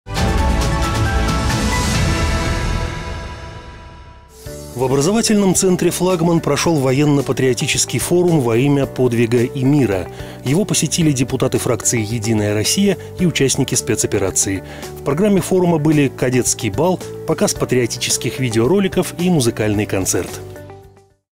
Во имя подвига и мира
В образовательном центре «Флагман» прошел военно-патриотический форум «Во имя подвига и мира». Его посетили депутаты фракции «Единая Россия» и участники спецоперации. В программе форума были «Кадетский бал», показ патриотических видеороликов и музыкальный концерт.